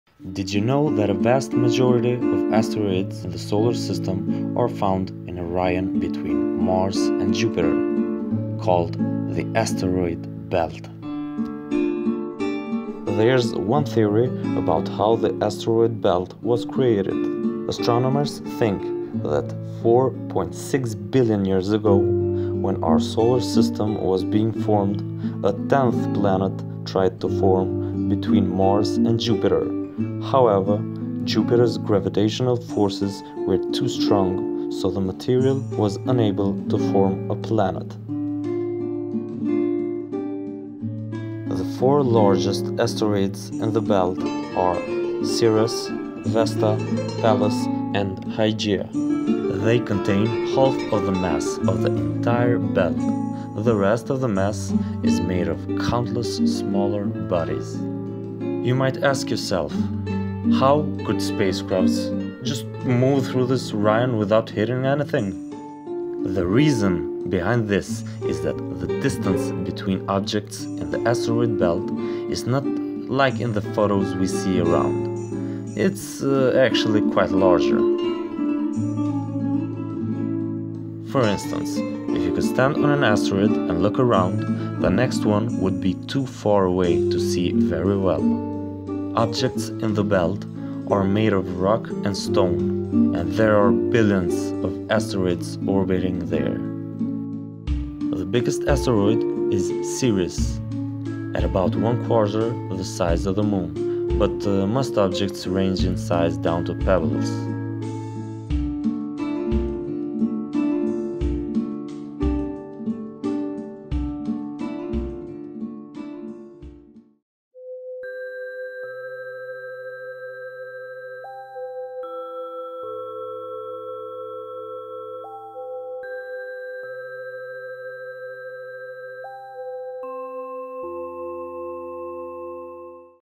The Asteroid Belt Between Mars and Jupiter
In this video, you can learn about the asteroid belt, a region between Mars and Jupiter where the vast majority of asteroids orbit the Sun.

Cosmoknowledge is an astronomy channel on the internet.